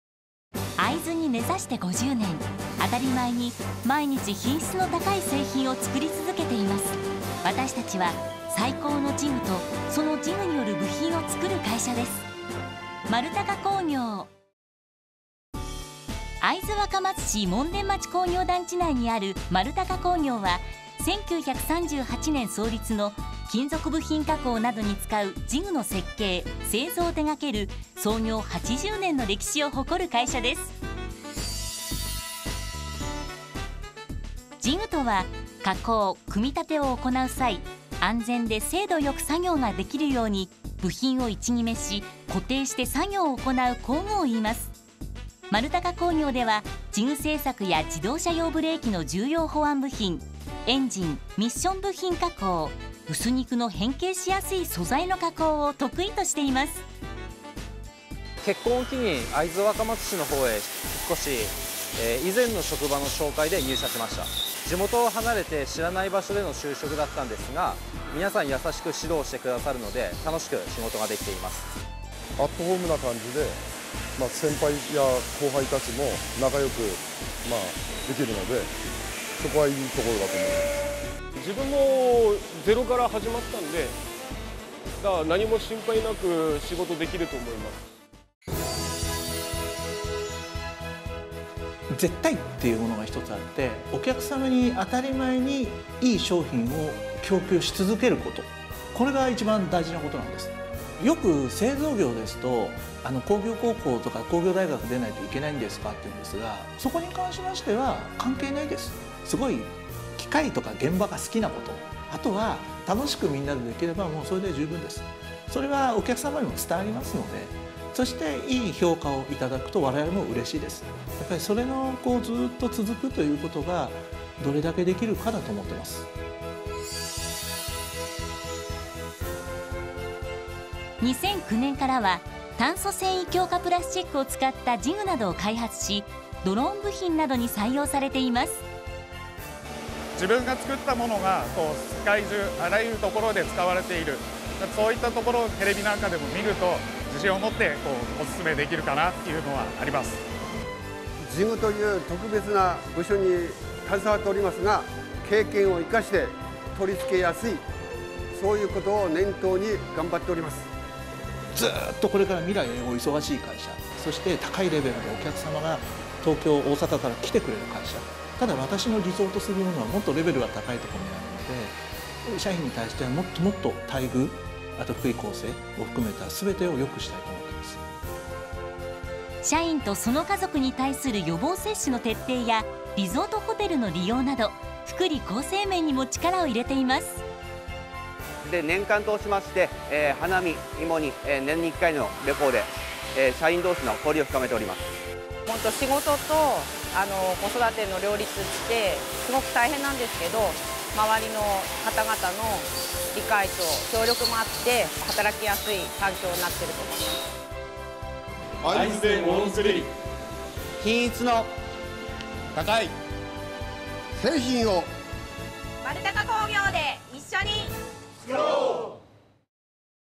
ふくしまのしごと、みてみよう。～「丸隆工業株式会社」
下町ロケットの東京 大田区にて発祥し、会津若松市にて50年。
大手メーカーの1次下請（Tier1）として福島県から東京を含めた関東、名古屋地区、大阪へカンバン納品を行う顔と、設計・製作を行う治具メーカーとしての顔、これからの素材であるCFRP（炭素繊維強化プラスチック）を成形・加工できる新たな顔を含めて　3つのスタイルを相互に関連させることで「お客様に喜んで頂く」こと「安心して社員に働いて頂く」ことを社是として これからの50年も更なる発展を目指していきます。